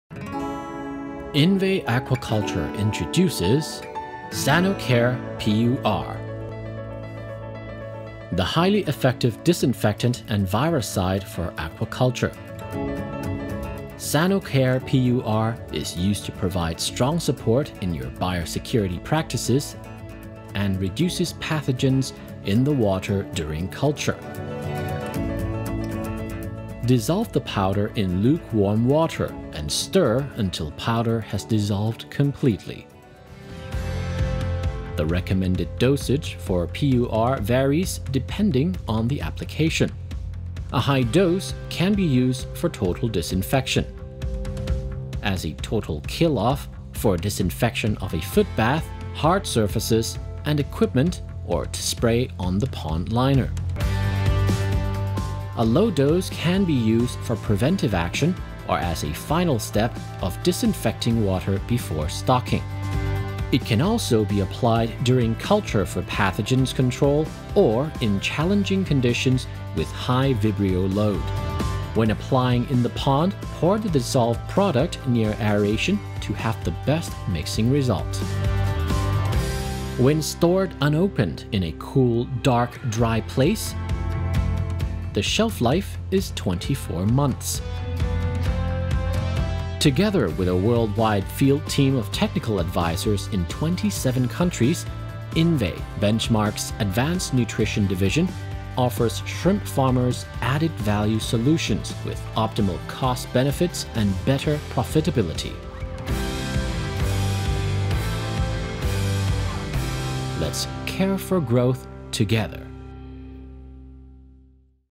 Sanocare® PUR by INVE Aquaculture
Guide on how to correctly use our Sanocare PUR product on your farm.

Sanocare® PUR is a highly disinfectant and virucide for aquaculture. This product is used to provide strong support in your biosecurity practices and reduces pathogens in the water during culture.